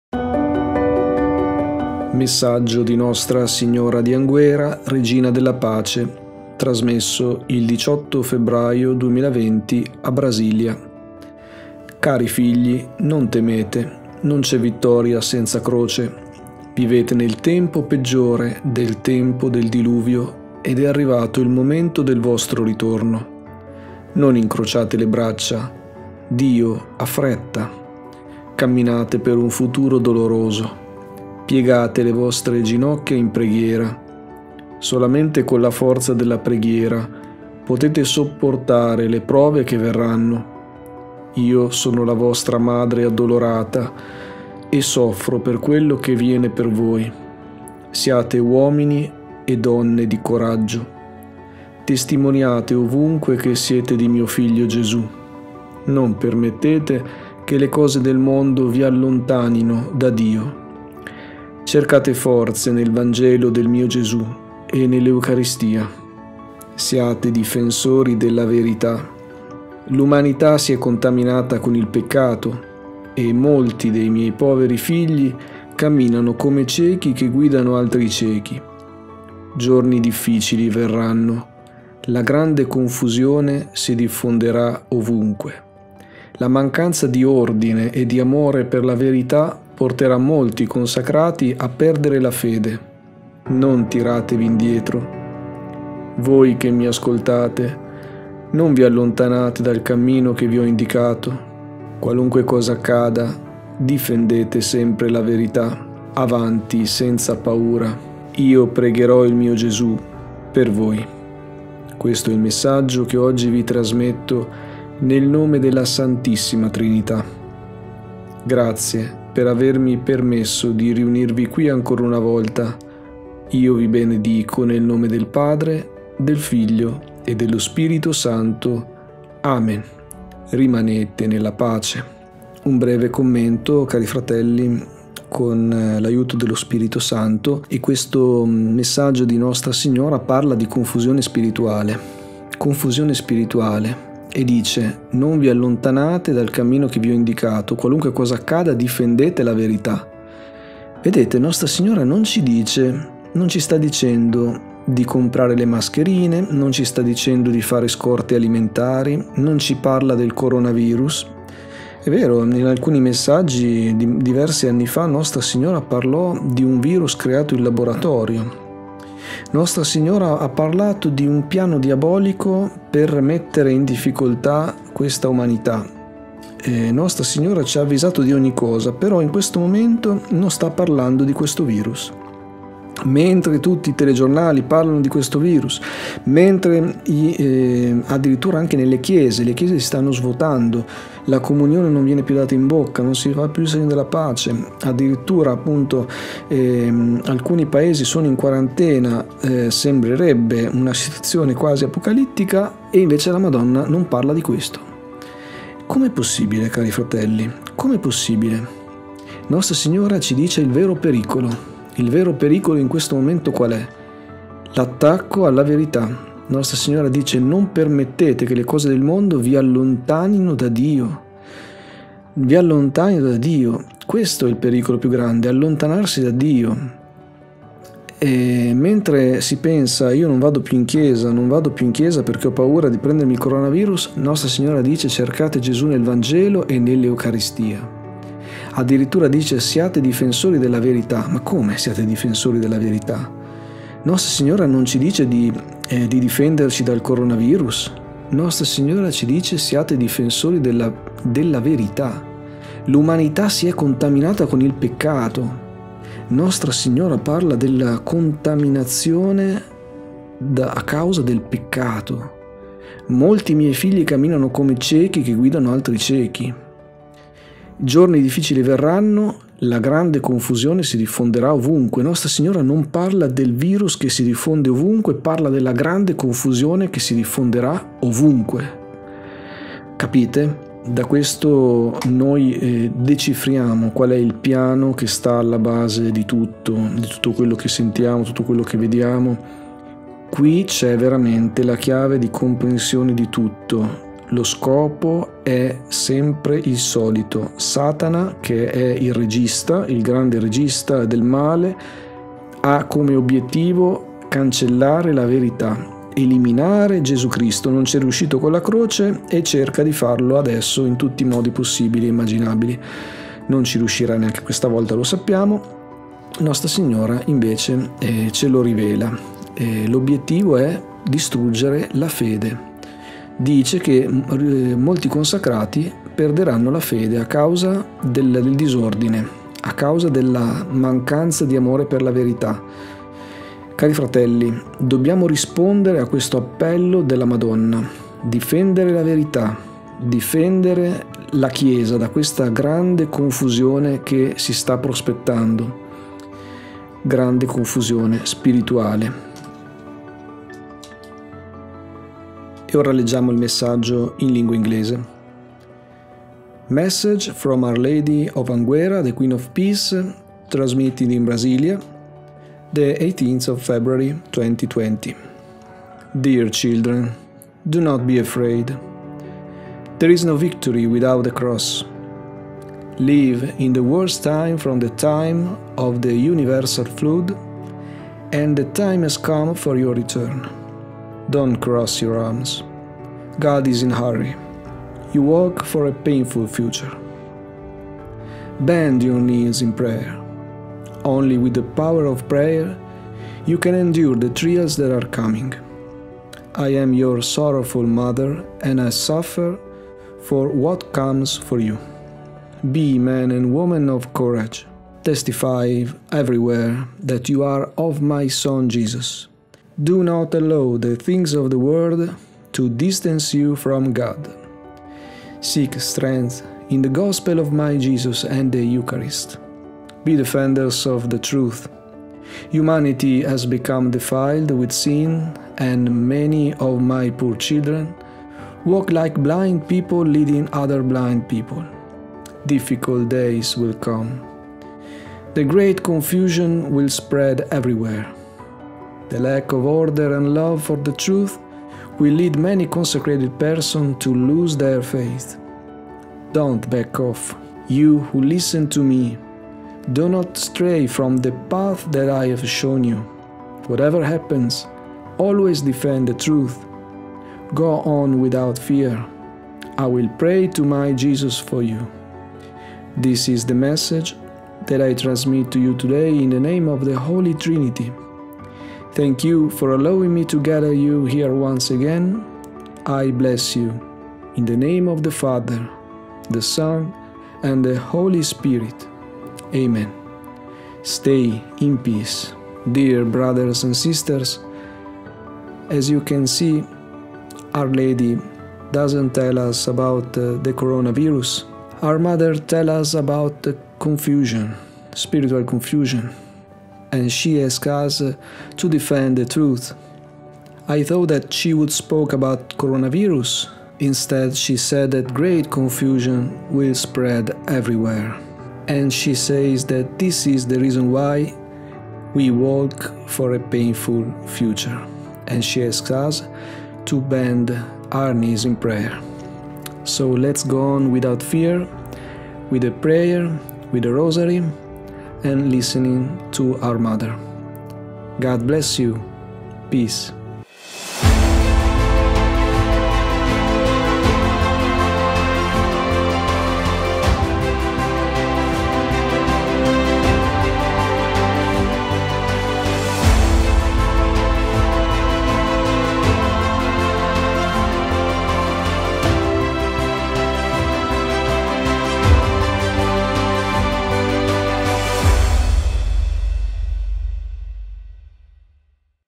Si diffonderà ovunque | Anguera 18/2/2020 - ITA/ENG
4.929 – Messaggio di Nostra Signora Regina della Pace, a Brasília, DF, trasmesso il 18/02/2020

Cari figli, non temete. Non c'è vittoria senza croce. Vivete nel tempo peggiore del tempo del Diluvio ed è arrivato il momento del vostro ritorno. Non incrociate le braccia. Dio ha fretta. Camminate per un futuro doloroso. Piegate le vostre ginocchia in preghiera. Solamente con la forza della preghiera potete sopportare le prove che verranno. Io sono la vostra Madre Addolorata e soffro per quello che viene per voi. Siate uomini e donne di coraggio. Testimoniate ovunque che siete di Mio Figlio Gesù. Non permettete che le cose del mondo vi allontanino da Dio. Cercate forze nel Vangelo del Mio Gesù e nell'Eucaristia. Siate difensori della verità. L'umanità si è contaminata con il peccato e molti dei Miei poveri figli camminano come ciechi che guidano altri ciechi. Giorni difficili verranno. La grande confusione si diffonderà ovunque. La mancanza di ordine e di amore per la verità porterà molti consacrati a perdere la fede. Non tiratevi indietro. Voi che Mi ascoltate: non vi allontanate dal cammino che vi ho indicato. Qualunque cosa accada, difendete sempre la verità. Avanti senza paura. Io pregherò il Mio Gesù per voi. Questo è il messaggio che oggi vi trasmetto nel nome della Santissima Trinità. Grazie per averMi permesso di riunirvi qui ancora una volta. Io vi benedico nel nome del Padre, del Figlio e dello Spirito Santo. Amen. Rimanete nella pace.

4.929 - Message from Our Lady Queen of Peace, in Brasília, DF, sent on 18/02/2020

Dear children, do not be afraid. There is no victory without a cross. Live in the worst time from the time of the Universal Flood and the time has come for your return. Don't cross your arms. God is in a hurry. You Walk for a painful future. Bend your knees in prayer. Only with the power of prayer you can endure the trials that are coming. I am your Sorrowful Mother and I suffer for what comes for you. Be men and women of courage. Testify everywhere that you are of My Son Jesus. Do not allow the things of the world to distance you from God. Seek strength in the Gospel of My Jesus and in the Eucharist. Be defenders of the truth. Humanity has become defiled with sin and many of My poor children walk like blind people leading other blind people. Difficult days will come. The great confusion will spread everywhere. The lack of order and love for the truth will lead many consecrated persons to lose their faith. Don't back off. You who listen to Me: do not stray from the path I have shown you. Whatever happens, always defend the truth. Go on without fear. I will pray to My Jesus for you. This is the message that I transmit to you today in the name of the Holy Trinity. Thank you for allowing Me to gather you here once again. I bless you in the name of the Father, the Son and the Holy Spirit. Amen. Stay in peace.